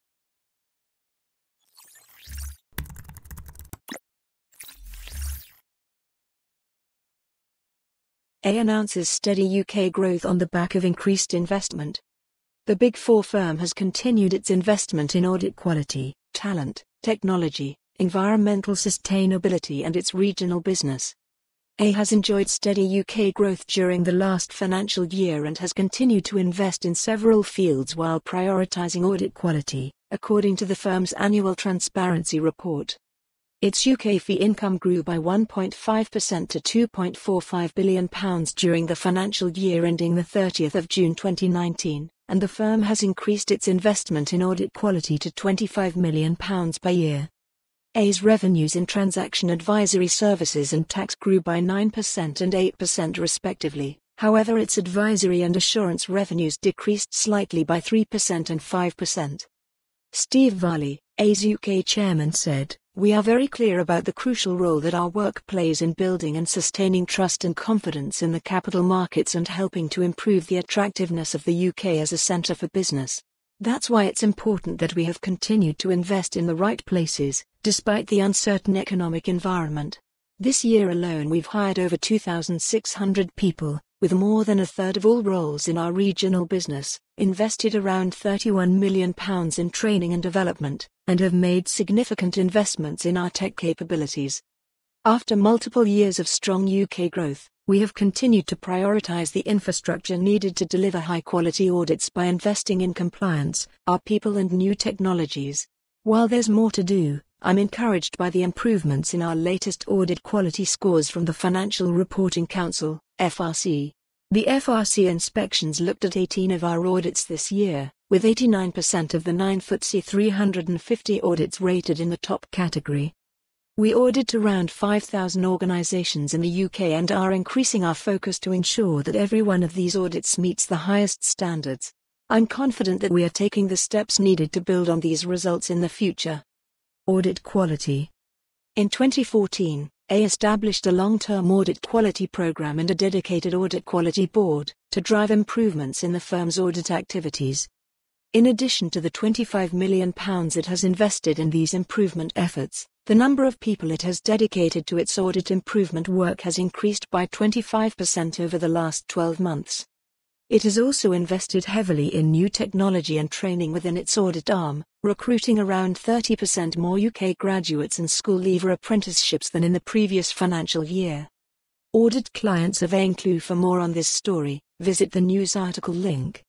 EY announces steady UK growth on the back of increased investment
Reported today on Accountancy Age

The Big Four firm has continued its investment in audit quality, talent, technology, environmental sustainability and its regional business.

EY has enjoyed steady UK growth during the last financial year and has continued to invest in several fields while prioritising audit quality, according to the firm's annual transparency report.

Its UK fee income grew by 1.5% to £2.45 billion during the financial year ending 30 June 2019, and the firm has increased its investment in audit quality to £25m per year.

EY's revenues in Transaction Advisory Services and Tax grew by 9% and 8% respectively, however its Advisory and Assurance revenues decreased slightly by 3% and 5%.

Steve Varley, EY's UK Chairman said: "We are very clear about the crucial role that our work plays in building and sustaining trust and confidence in the capital markets and helping to improve the attractiveness of the UK as a centre for business. That's why it's important that we have continued to invest in the right places, despite the uncertain economic environment. This year alone we've hired over 2,600 people, with more than a third of all roles in our regional business, invested around £31m in training and development, and have made significant investments in our tech capabilities.

"After multiple years of strong UK growth, we have continued to prioritise the infrastructure needed to deliver high quality audits by investing in compliance, our people and new technologies. While there's more to do, I'm